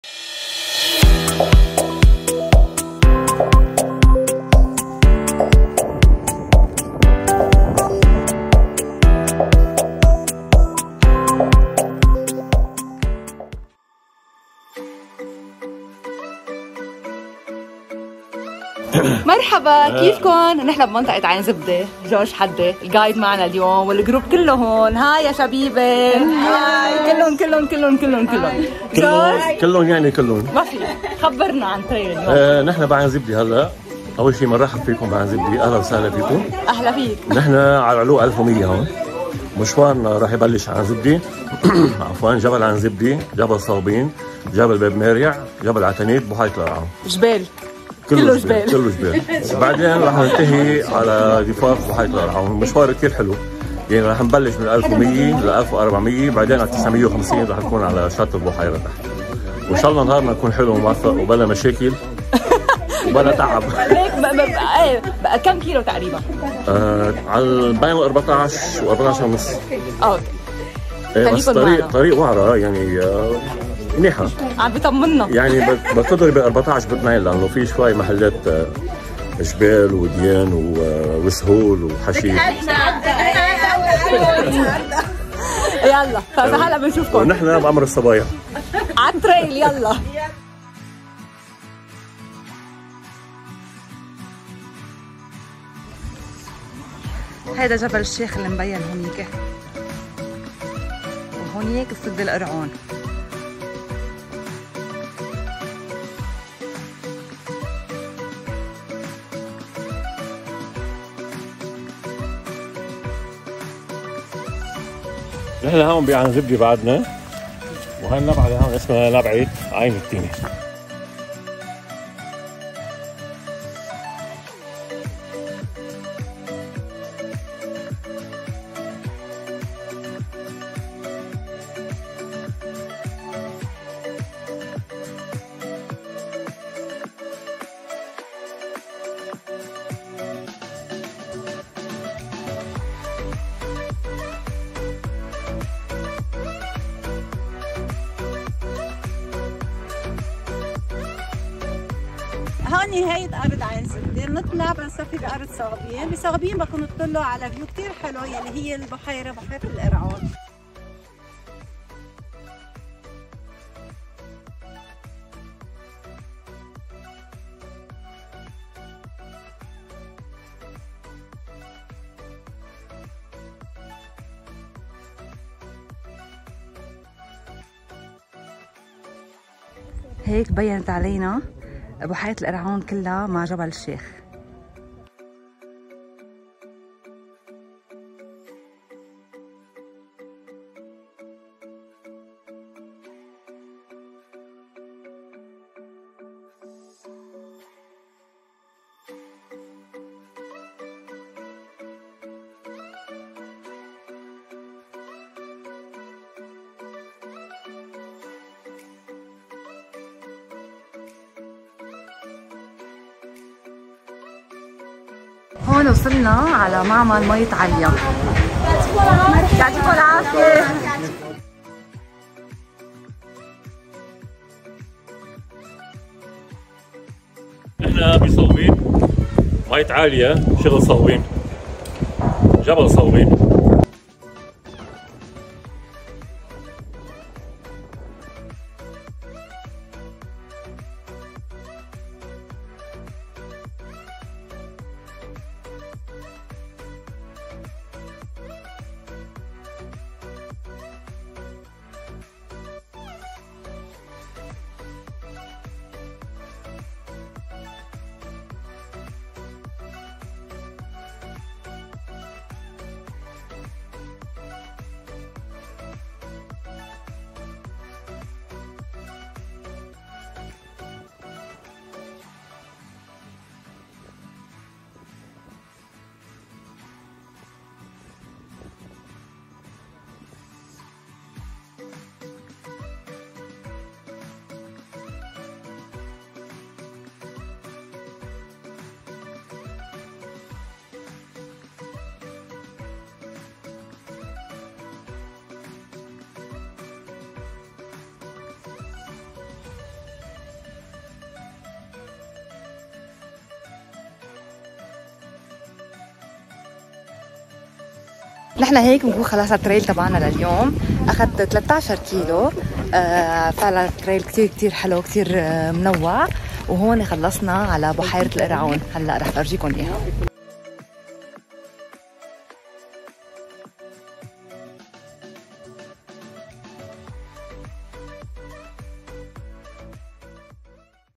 Ain Zebdeh
Today we went to west Bekaa, Ain Zebdeh is a very nice village with a great trail, we walked till we arrived to Beb Mereh, at the Qaraoun Lake, passing by Alia water in Saghbine.
Great sceneries.